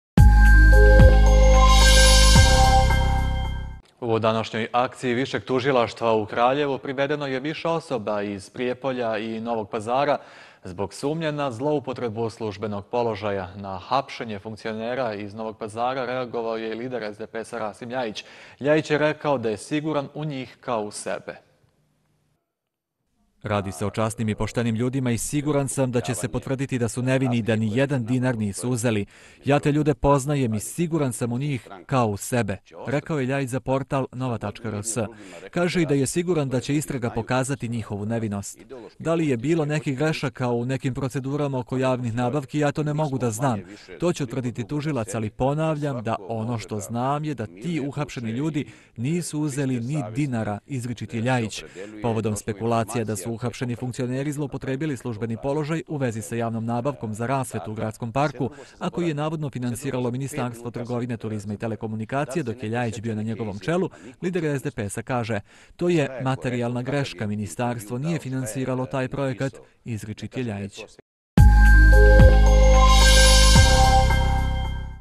Ljajić o hapšenju funkcionera: Siguran sam u njih kao u sebe
U današnjoj akciji Višeg tužilaštva u Kraljevu privedeno je više osoba iz Prijepolja i Novog Pazara zbog sumnje na zloupotrebu službenog položaja. Na hapšenje funkcionera reagovao je i lider SDPS-a Rasim Ljajić. Ljajić je rekao da je siguran u njih kao u sebe.